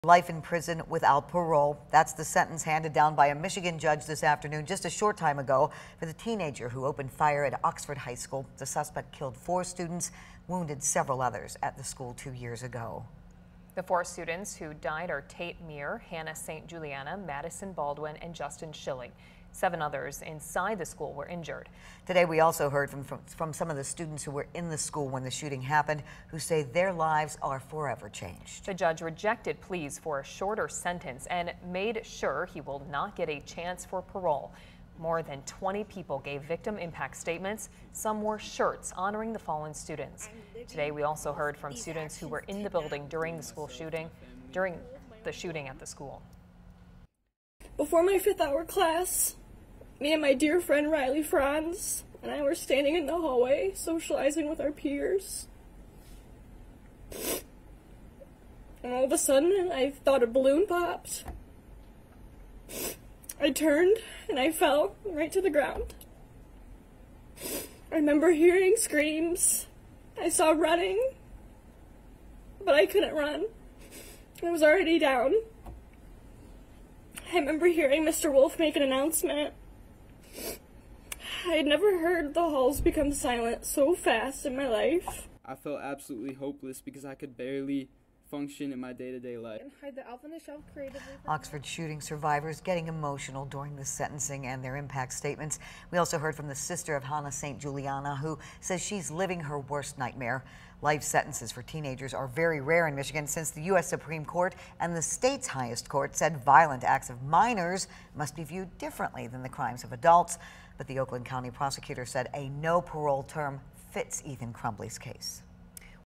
Michigan school shooter gets life in prison for Oxford High School Attack
A judge in suburban Detroit heard hours of anguish from families and surviving victims of the Oxford school shooting before making the ruling.